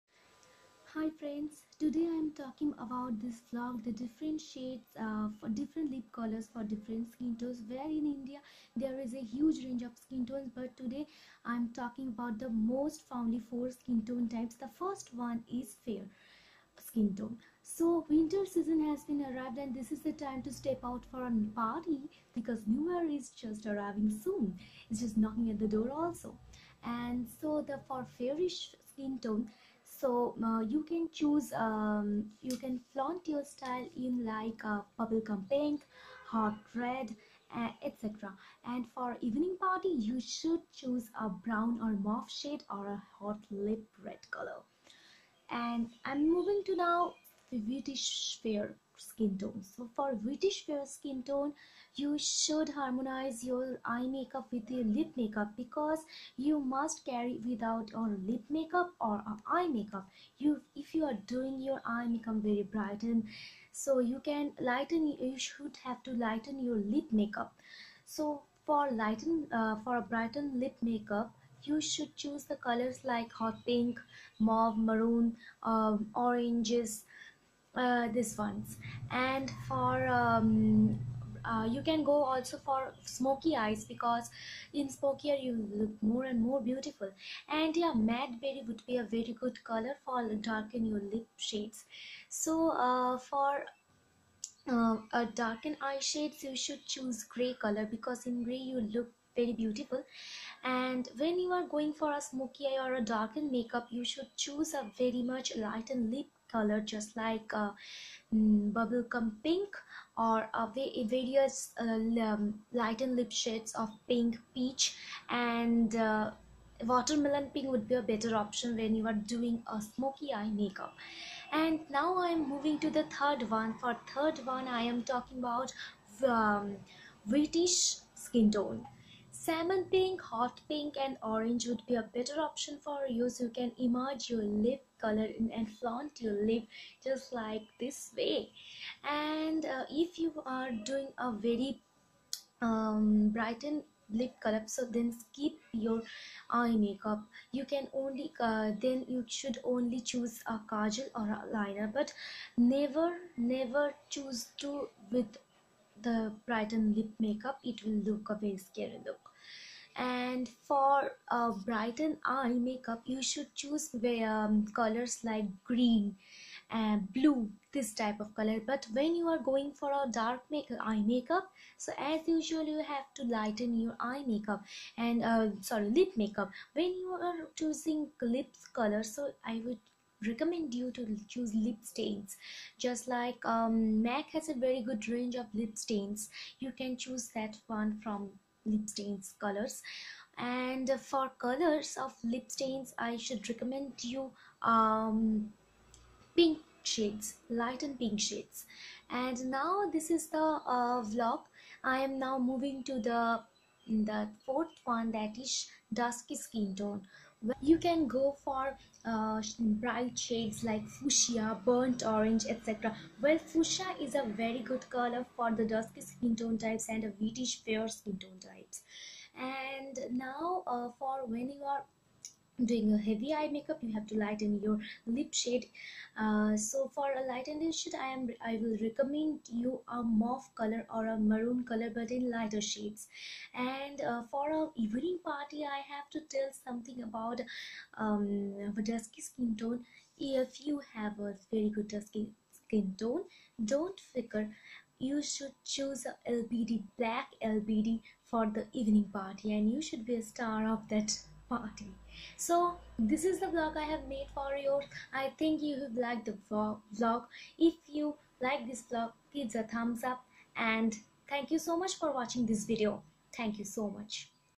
Different Lip colours for different SKIN TONE..
Fair tone bubblegum pink, hot red,peach,mauve
Wheatish fair--- pink fuschia, coral,plum
Wheatish --- watermelon pink, hot pink, berry pink
Dusky tone burnt orange, pink shade like hot pink